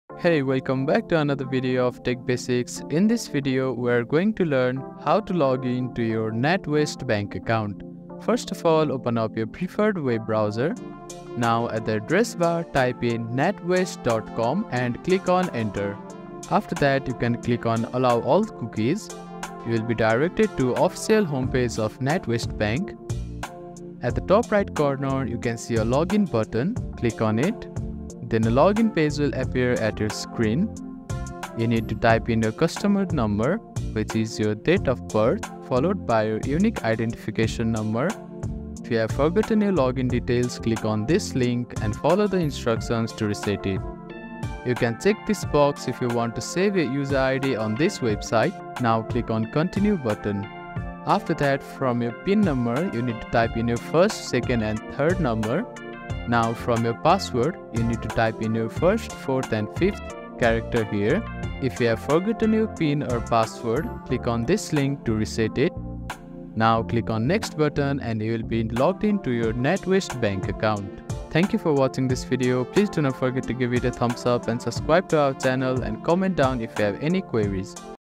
How to Login Natwest Bank Account 2024?
Confused about logging in to your Natwest bank account online? You're not alone! This video will be your key to navigating Natwest's secure login system. We'll guide you through every step, from visiting the official Natwest website to entering your credentials and securely accessing your account.  Whether you're a new online banking user or just need a refresher, this video will equip you with the confidence to log in and manage your finances anytime, anywhere.
We'll break down each step clearly, including using your customer number or card number, PIN, password, and even handling one-time passcodes for an extra layer of security. By the end of this video, you'll be logging in to your Natwest account like a pro!